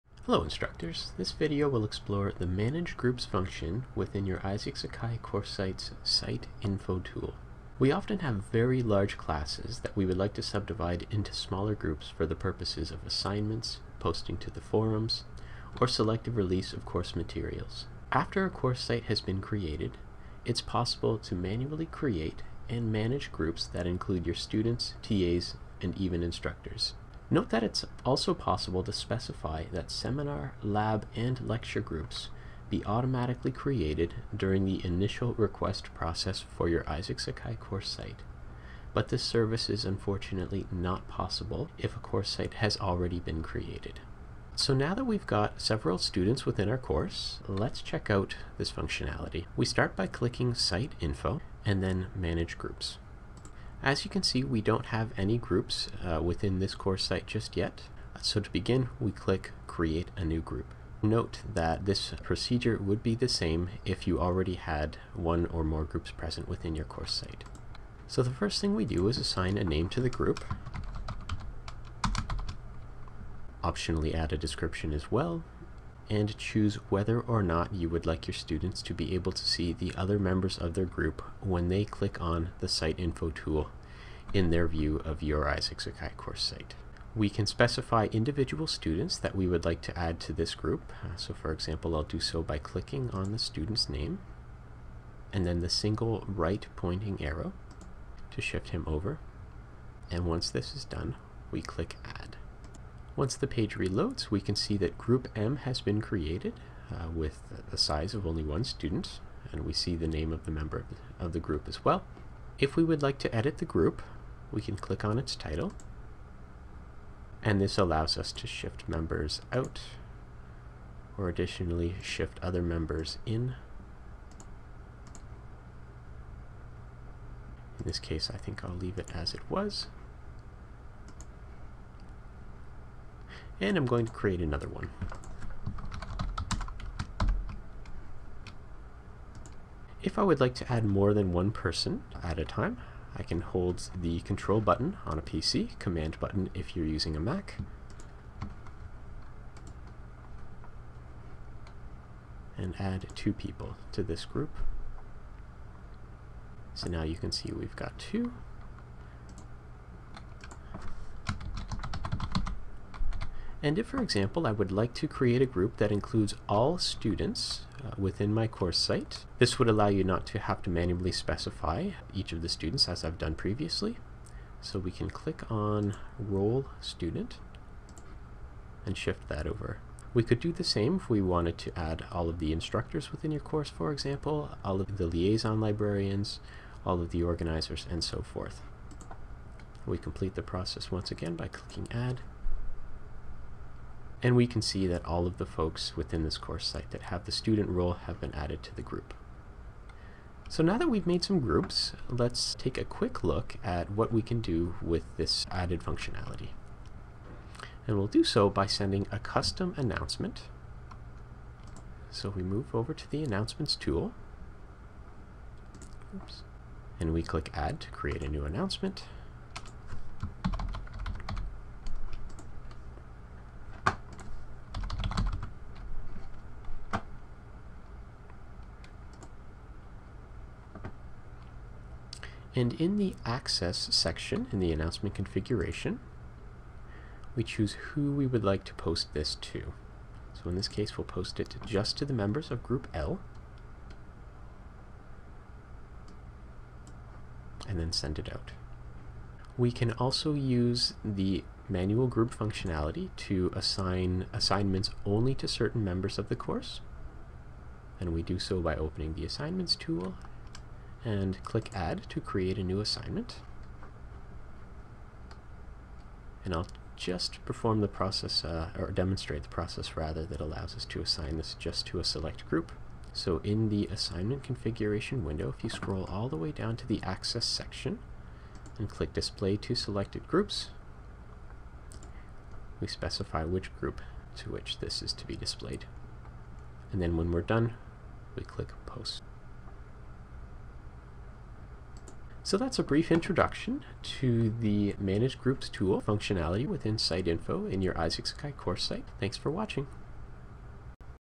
Isaak-Sakai's Manage Groups feature
Isaak-Sakai's Manage Groups feature, which is accessed via the Site Info tool, is useful for organizing your class roster into subsets for the purpose of directing content, messages, or assessments at specific users.

Hosted by Marc Breschuk, Educational Technology Support Assistant, Centre for Pedagogical Innovation. If you would like assistance with the Manage Groups feature or any of Isaak-Sakai's tools, please contact Marc